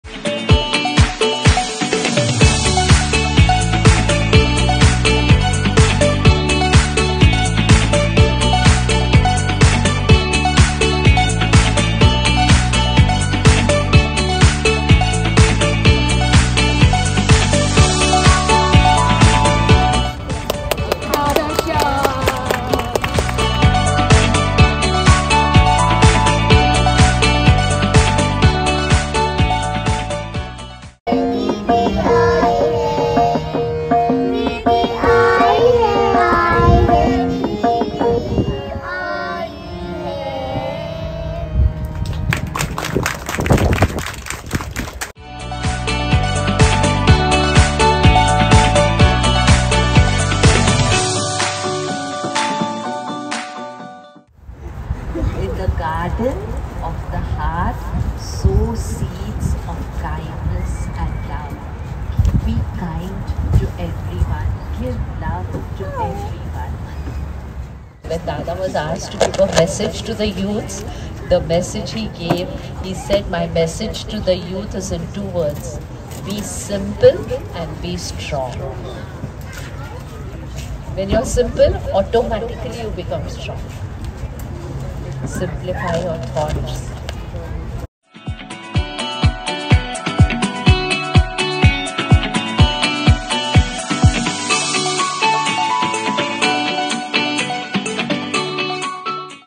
Energize Yourself | Youth Meet | Didi Krishna | Texas | April 30, 2024
Energizing glimpses from the Youth Meet with Didi Krishna at Bonnie Wenk Park, Texas - April 30, 2024